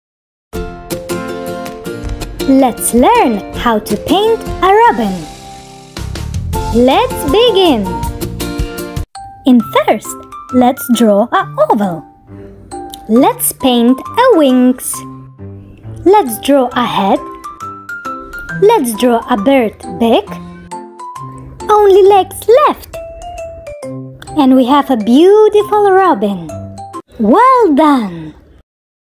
How to draw a Robin
Educational beautiful video teaches kids how to draw a Robin